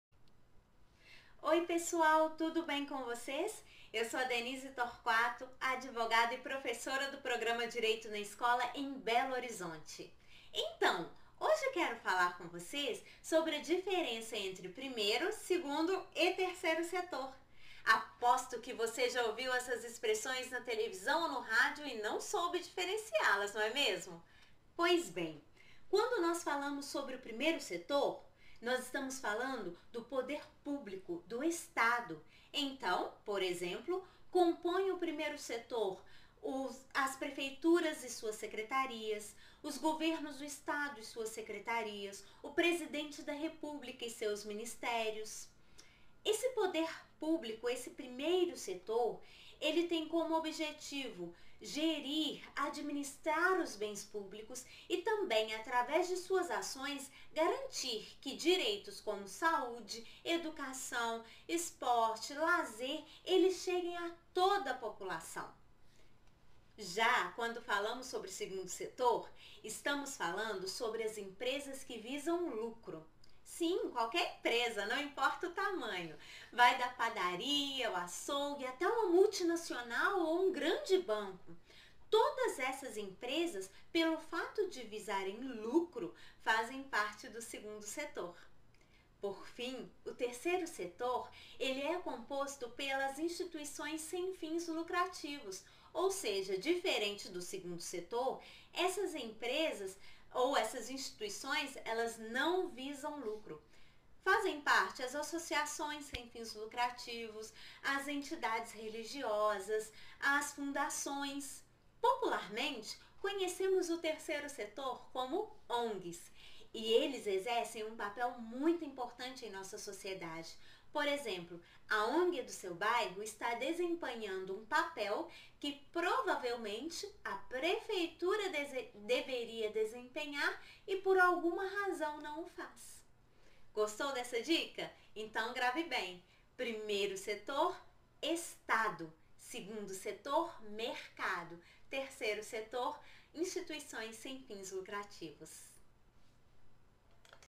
ONG ? Empresas? Entenda a diferença entre primeiro, segundo e terceiro setor
A professora @denisetorquato1 ensina a diferença entre,  primeiro,  segundo e terceiro setor.
no Brasil ainda precisamos avançar e participar mais da construção da nação,  tudo começa com o conhecimento do Direito.